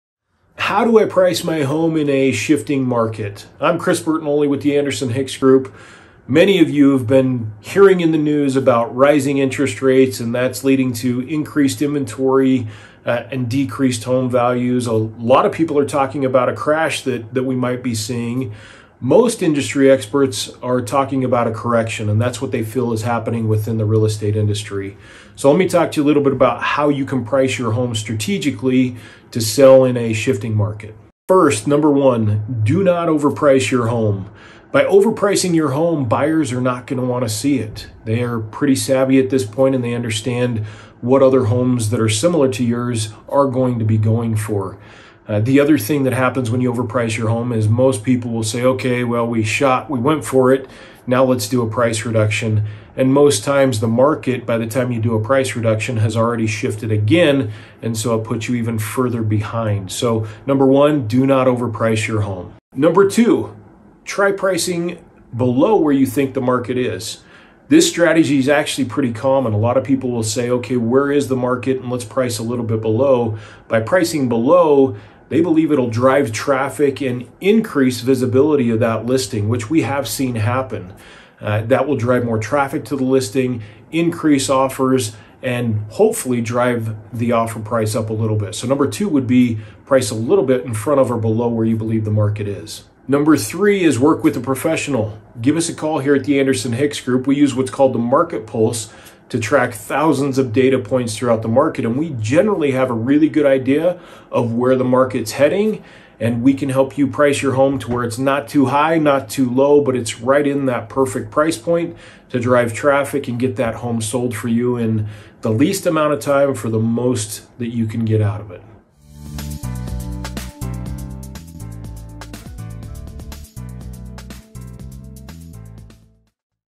What Sellers Should Do in This Market
Sellers: News of our housing market’s death has been greatly exaggerated. It’s true that the exploding housing prices we saw in the last two years are behind us, but we aren’t heading for a crash—instead, our market is correcting. This means you can still receive top dollar for your home, but you have to be smart about it. That’s why I’m sharing a few tips to help you price your home the right way in a correcting market. To learn more, check out this video.

Ready to be a homeowner?

Anderson Hicks Group
Keller Williams Realty East Idaho
3525 Merlin Dr
Idaho Falls, ID 83404